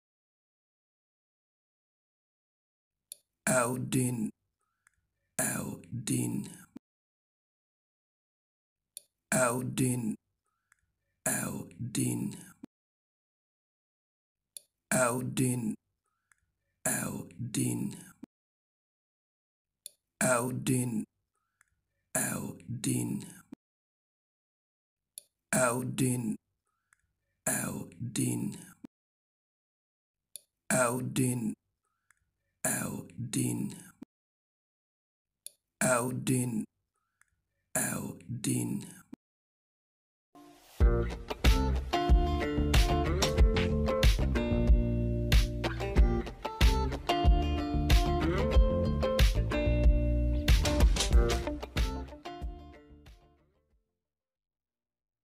How to pronounce Eldin
Eldin, an alternative transliteration of Ad-Din ("faith", "religion"), is a given name and a surname.